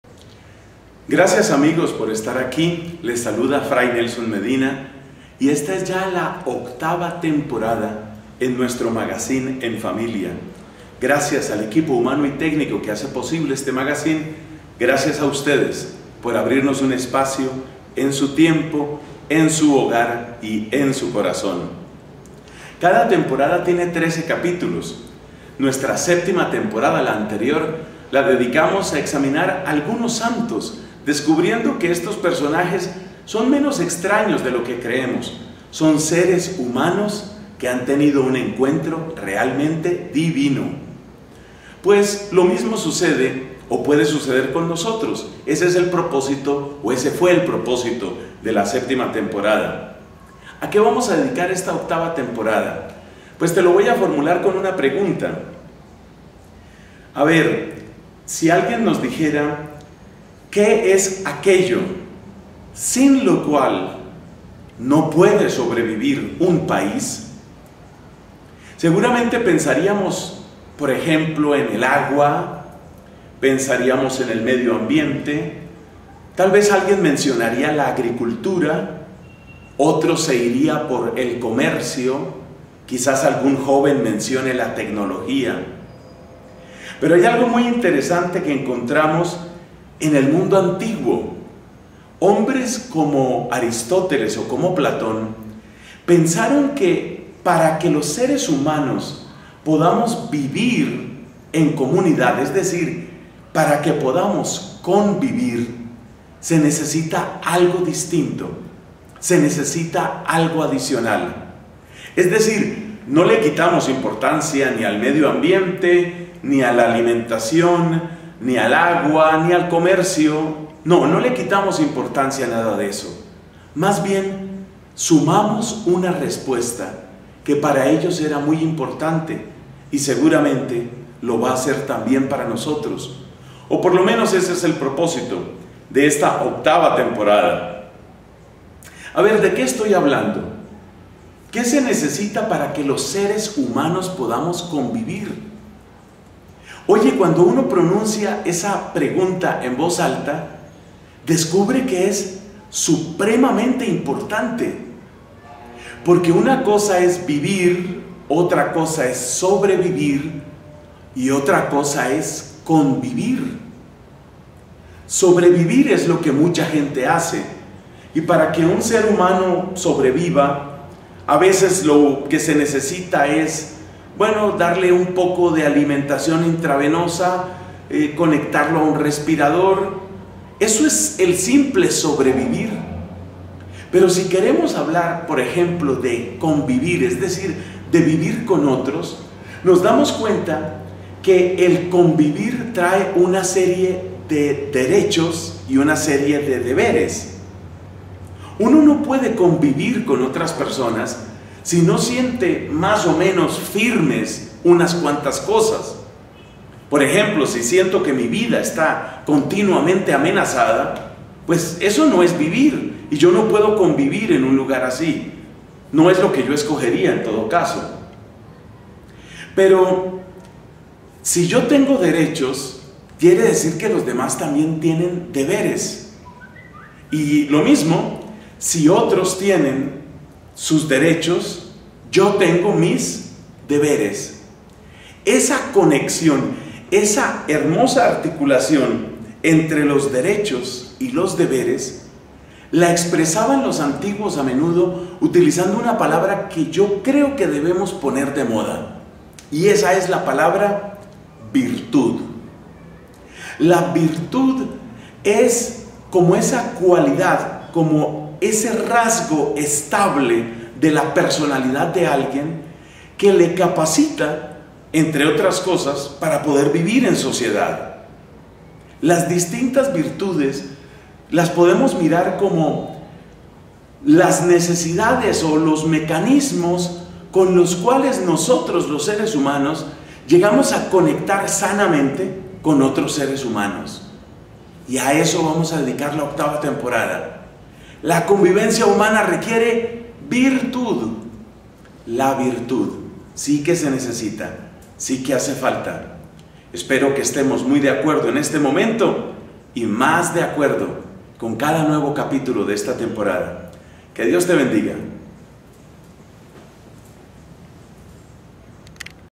ENFAMILIA 08-01, Vivir, sobrevivir y convivir
La virtud es necesaria para la convivencia. La vida en sociedad reclama el comportamiento estable que es propiod e las personas virtuosas.